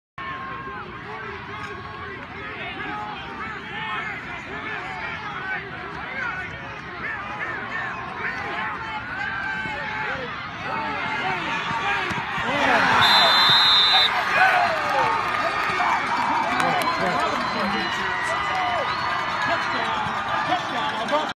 Ryan Barry finds Colin Robinson on a 20-yard hookup to put Algonquin up 13-7 in the 2Q.
By: Chris Kyne
Published on: November 4, 2016